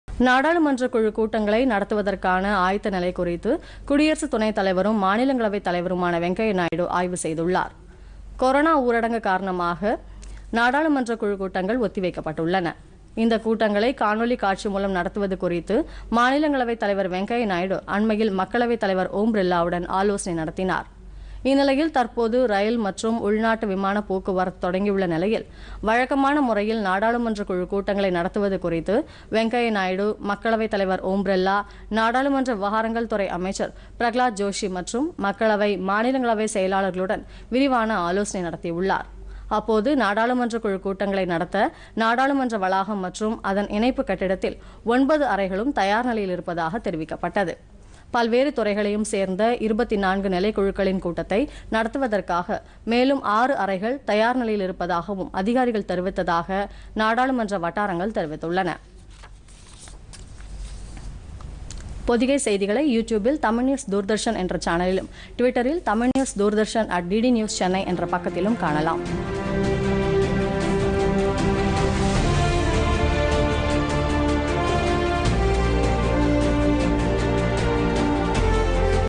நாடாளுமன்றக்குழுக் கூட்டம் பற்றி ஆலோசனை
நாடாளுமன்றக்குழுக் கூட்டங்களை நடத்துவது குறித்து மக்களவைத் தலைவர் ஓம் பிர்லா, மத்திய அமைச்சர் பிரகலாத் ஜோஷியுடன் குடியரசுத் துணைத் தலைவர் வெங்கய்யா நாயுடு ஆலோசனை